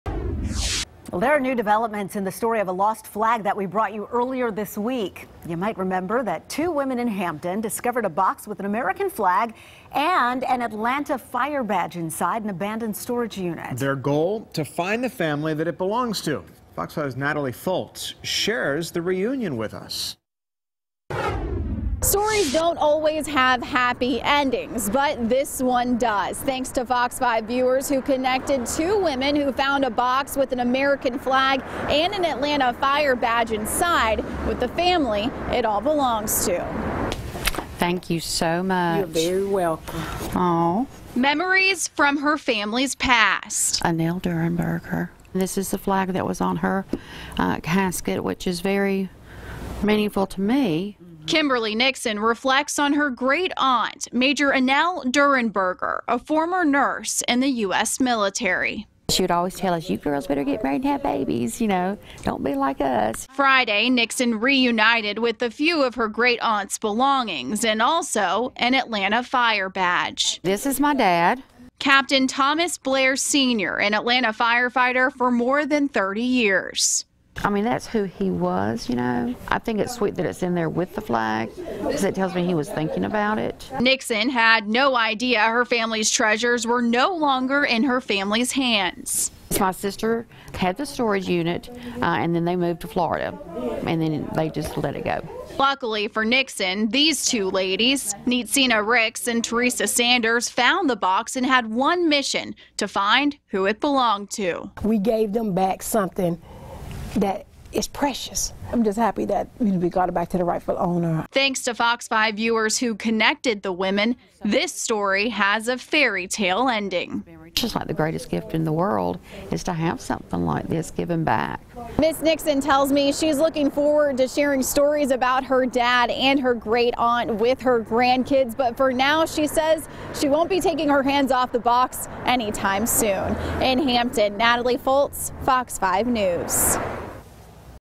Flag, Atlanta Fire badge reunited with family
After watching Tuesday's story, FOX 5 viewers helped connect Neetsinna Ricks and Theresa Sanders with the owner of the box found in the abandoned storage unit.

"This is the greatest blessing," Kimberly Nixon told FOX 5.

Inside the box, an Atlanta Fire badge and an American flag. Kimberly Nixon says those belong to her family.

"The badge is my dad's," Nixon said. "He was a Captain for more than 30 years, and the flag was put over my great Aunt's casket at her funeral. She was a nurse in the U.S. Military."

Ricks and Sanders told FOX 5 they had one mission-to find who the box belonged to.

"We gave them back something precious," Ricks said.

"I'm just so happy we found them," Sanders said.

Finding items left behind in storage units is pretty common, but two women who recently bought a unit at General Storage in Hampton, said they found something totally unexpected: a box with an American flag, an Atlanta Fire badge, a cross and even a coffee mug.

“We looked at each other like what have we found,” Neetsinna Ricks said.\

VIDEO REPORT: Atlanta firefighter memorial found

Neetsinna Ricks and Theresa Sanders are the co-owners of Sylvia’s Closet, a thrift store, in Hampton. Recently, they were the highest bidders for an auctioned off storage unit.

“We get the units and store items for our store,” Theresa Sanders said.

Sanders said when they opened the unit, it was jam packed. Once they went through it, they made the interesting discovery.

“We found a box... it’s a box for a fallen fireman in Atlanta. It has a cross that his wife or his mother would have been presented with,” Sanders said.

“We were both in shock,” Ricks said. “I mean why would somebody leave something like that in a storage unit.”

Whatever the circumstances, Ricks and Sanders said it doesn’t matter. They want to find the person or family to which the box belongs.

“If it was me or my family, I would want it back,” Sanders said.

FOX 5 News reached out to General Storage. They said they are working to track down the previous owner.

NEXT ARTICLE: Deputy helps high schooler with adorable traffic stop ‘promposal’